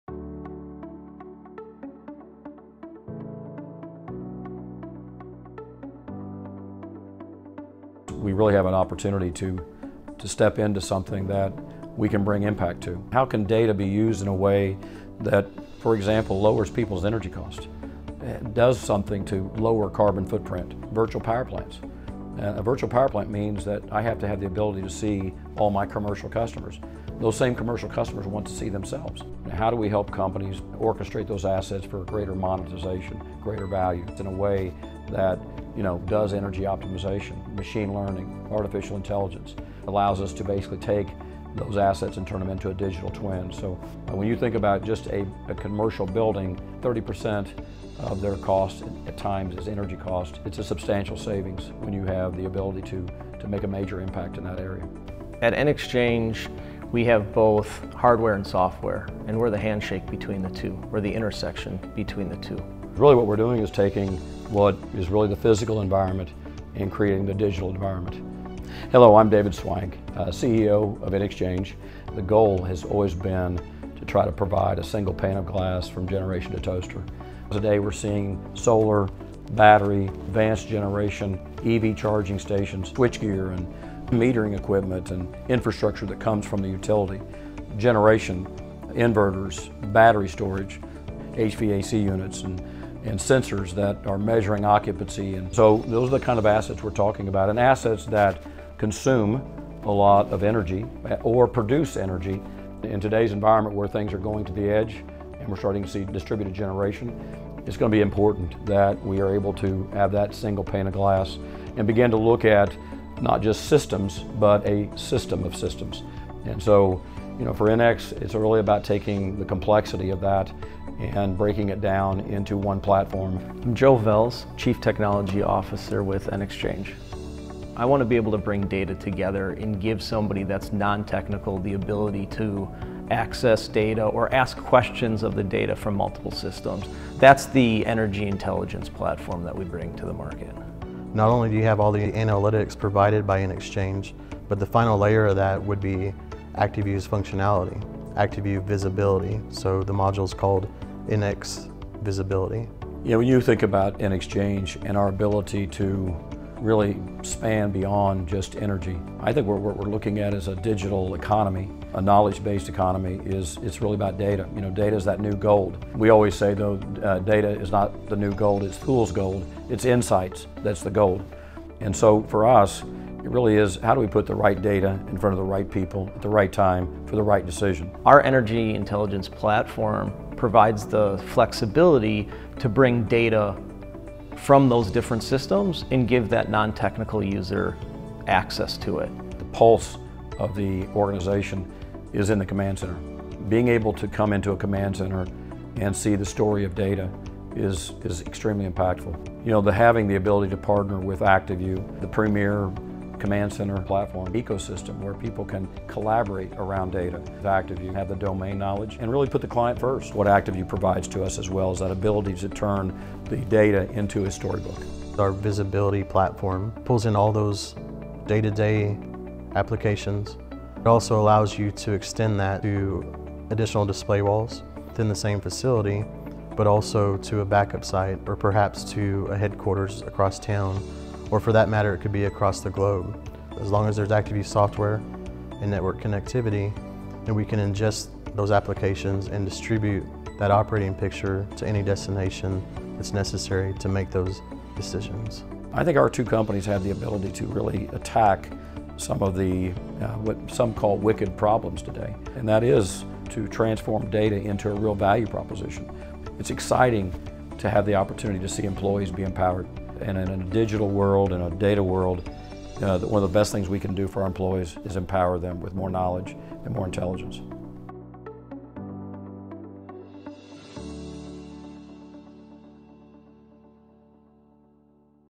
enx vis/ability - Next Gen Power Management
Integrated seamlessly for the control room with visability, EnXchange’s platform, enx vis/ability, empowers utilities, companies, airports, and more. Users of enx vis/ability collect a more detailed view of their operations than ever before, gain exclusive analytics, and easily share insights with teams in the control room, another facility, or in virtual command centers accessible anywhere.